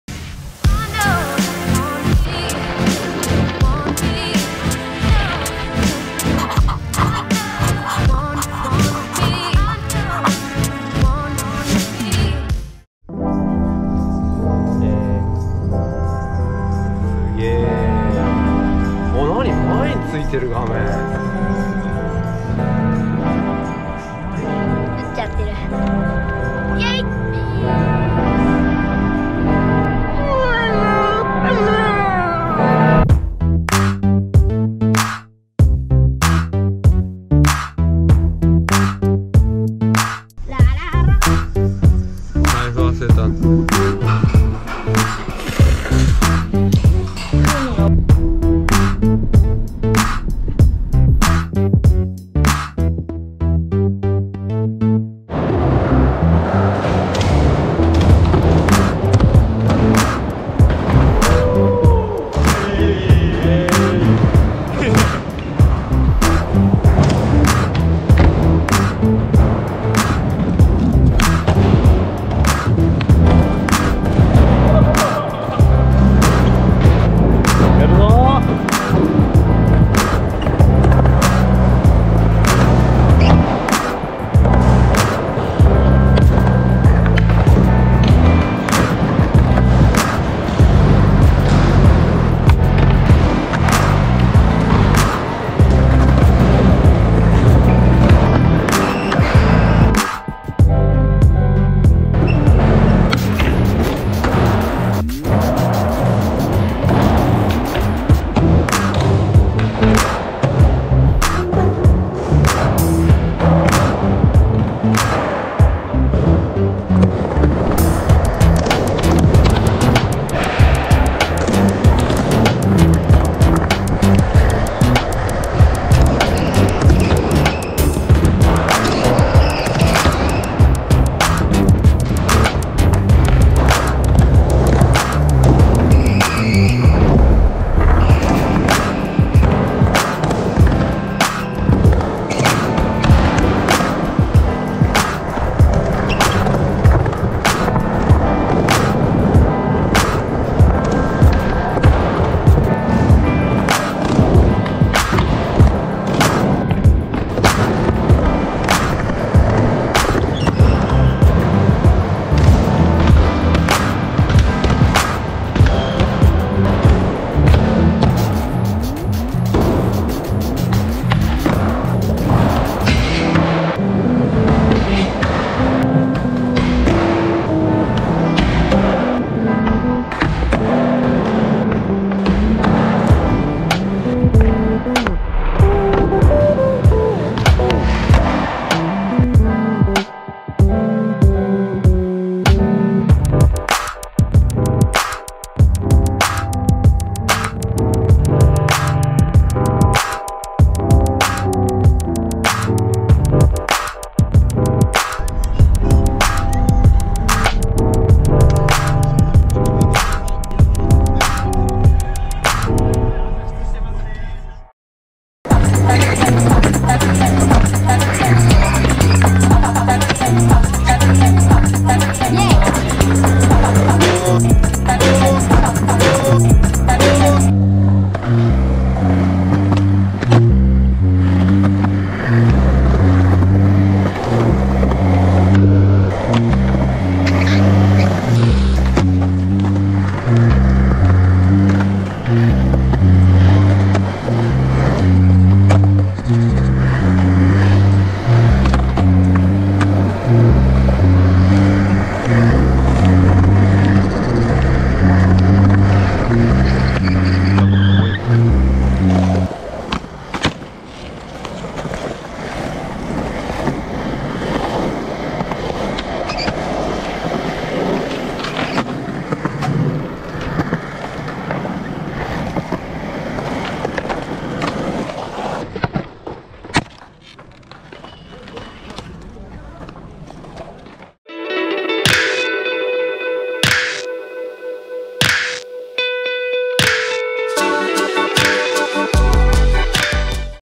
【小学生】マジで上手すぎるキッズスケーター達【スケボー】
はるばる北海道から友達が連れて来た小学生の激ウマキッズスケーター達と２日間滑って来ました〜
【1日目】
UNCHEINZ skate park
〒369-1304　埼玉県秩父郡長瀞町本野上363（フォレストサンズ長瀞内）
水曜～日曜 11:00～21:00（※最終受付20:00）
月曜・火曜  11:00～18:00（※最終受付18:00）
定休日 なし

【2日目】
大谷津運動公園スケートボードパーク
〒286-0845 千葉県成田市押畑９５２−３

彼らのINSTAGRAMも必見！

【WEBサイト】
▶︎壁田竜一オフィシャルサイト